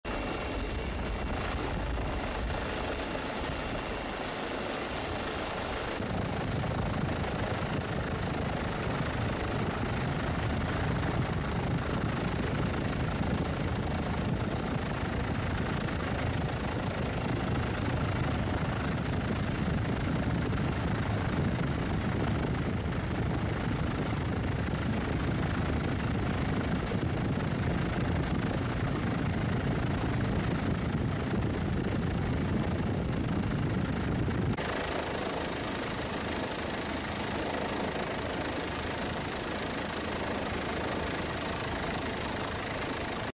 台風10号、4人不明の現場で捜索再開…宮崎
台風１０号に伴う土砂崩れで、４人と連絡が取れなくなった宮崎県椎葉村（しいばそん）の現場では８日、捜索活動が再開された（本社ヘリから）=大阪本社写真部　近藤誠撮影　2020年9月8日公開